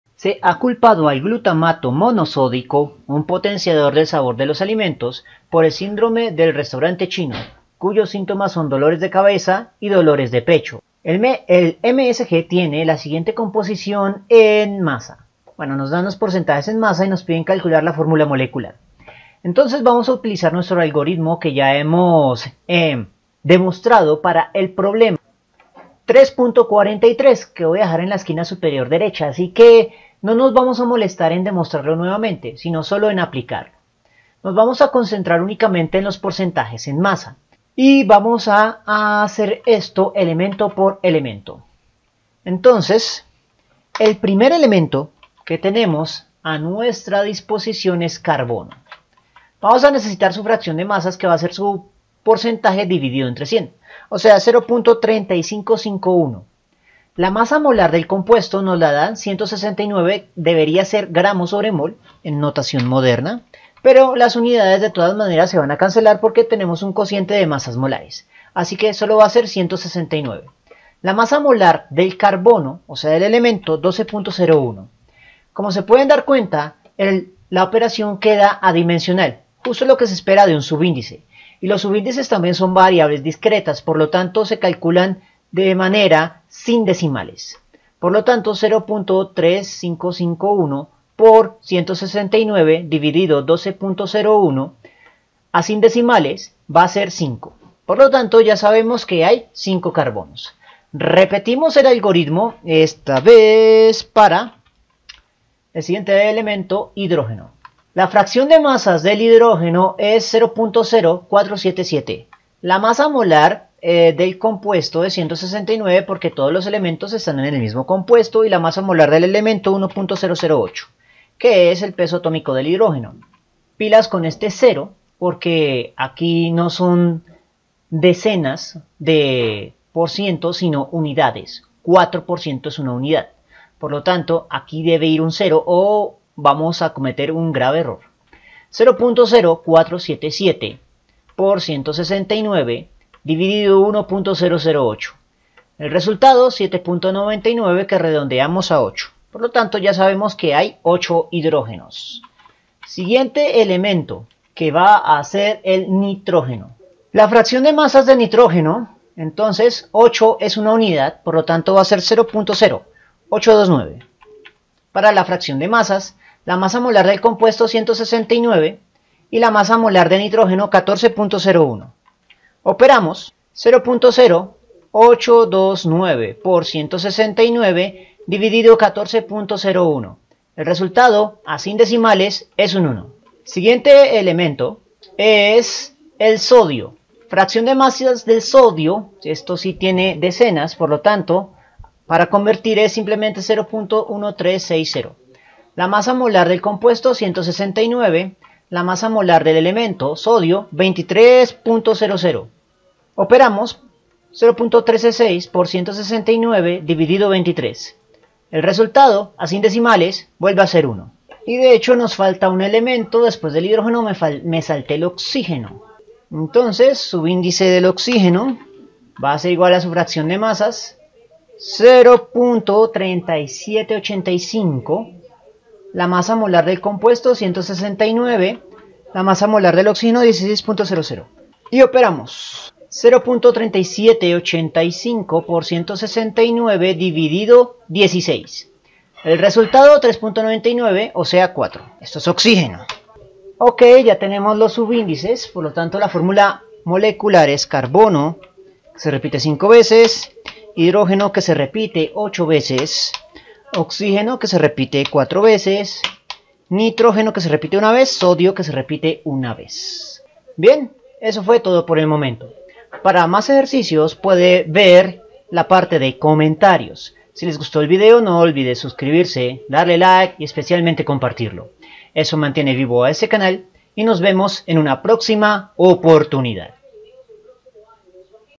Formula molecular de 35.51 % C 4.77 % H 37.85 % O 8.29 % N 13.60 % Na y masa molar 169 g
Mas ejercicios resueltos de química sobre del átomo químico:
Veremos cómo calcular los subíndices de una fórmula molecular con la fórmula empírica y la masa molar verdadera. Teórica atómica clásica, el átomo de los químicos.
Enunciado: (Química Chang10 problema 3.54) Se ha culpado al glutamato monosódico (MSG), un potenciador del sabor de los alimentos, por el "síndrome del restaurante chino", cuyos síntomas son dolores de cabeza y dolores de pecho. MSG tiene la siguiente composición en masa: 35.51 por ciento de C, 4.77 por ciento de H, 37.85 por ciento de O, 8.29 por ciento de N y 13.60 por ciento de Na. ¿Cuál es su fórmula molecular si su masa molar es de aproximadamente 169 g?

No olviden consultar nuestro web “ parte de nuestra serie de blogs de cienciasdejoseleg, donde encontrarán
Fundamentos teóricos y referencias bibliográficas del átomo químico: